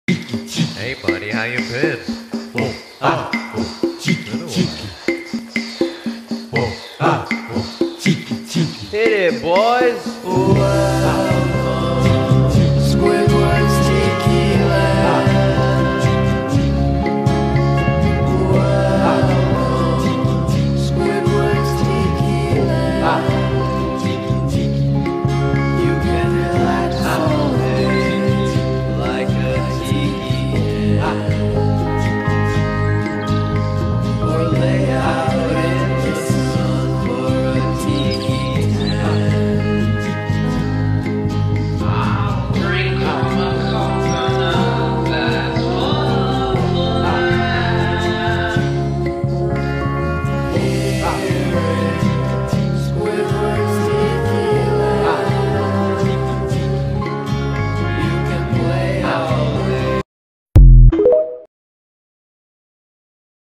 Brick Build: Squidward!
Well this was another dollar tree find! It’s great! Can’t wait to build the rest!!!